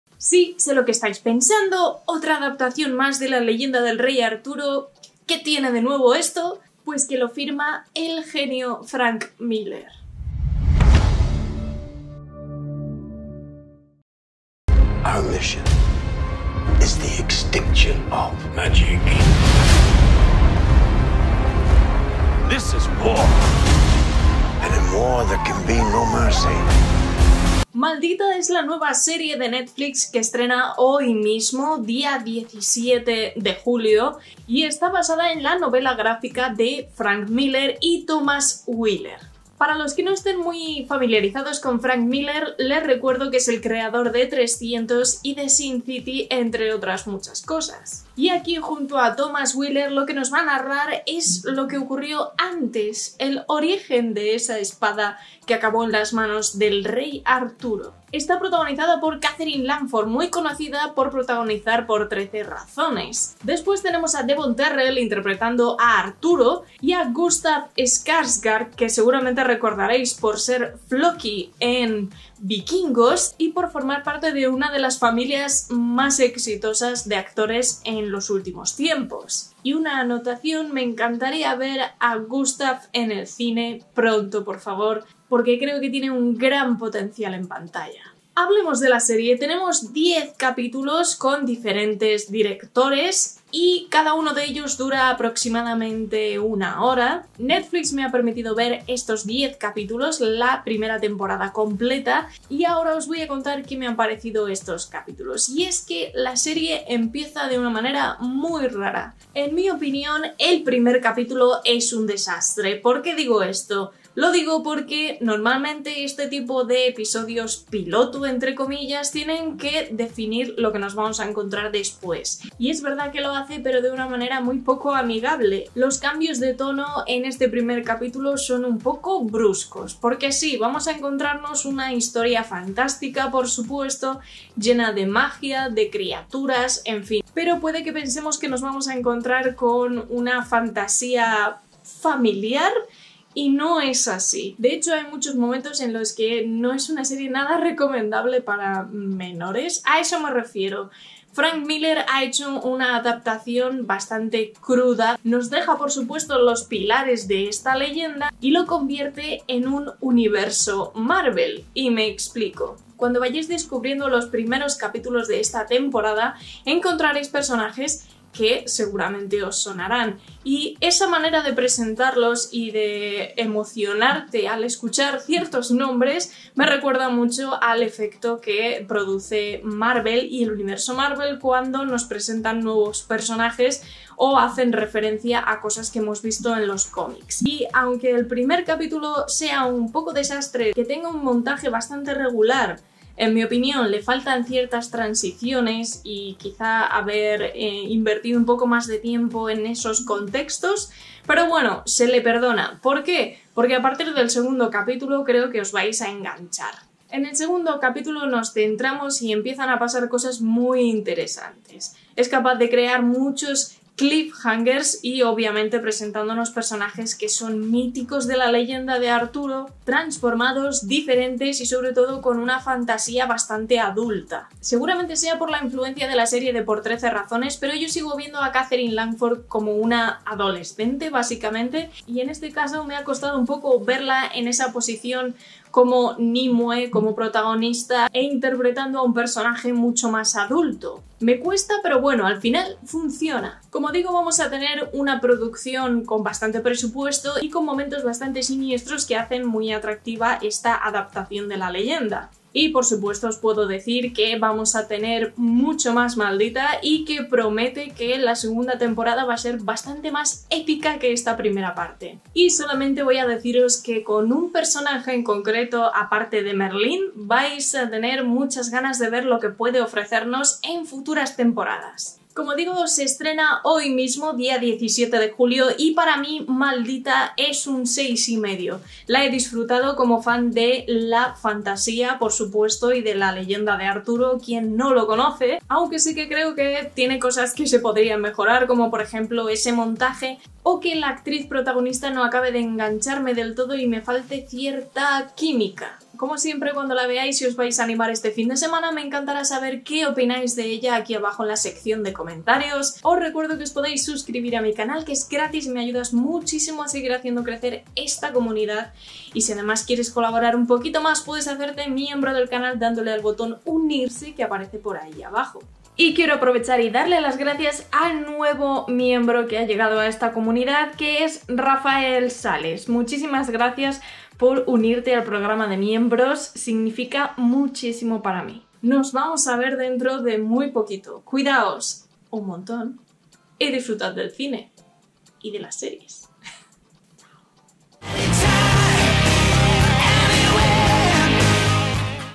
Crítica - 'Maldita' Primera temporada / Netflix / (Sin spoilers)
Mi opinión / crítica / reseña de la nueva primera temporada de la serie de Netflix 'Maldita' creada por Frank Miller y Thoman Wheeler y protagonizada por Katherine Langford, Devon Terrell y Gustaf Skarsgard.